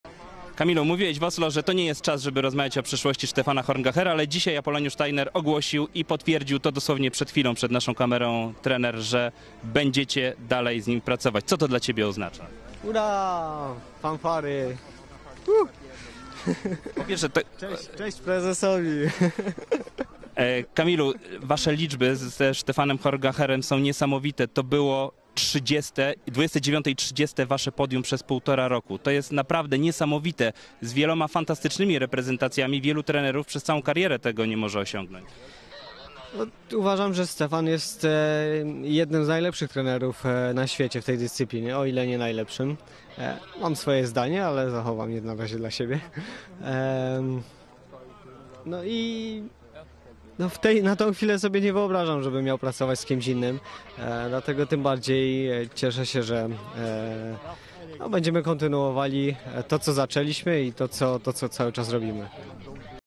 Niecodzienna reakcja Kamila Stocha na wiadomość o pozostaniu Horngachera na stanowisku trenera
Stefan Horngacher przez kolejne sezony będzie trenerem naszej reprezentacji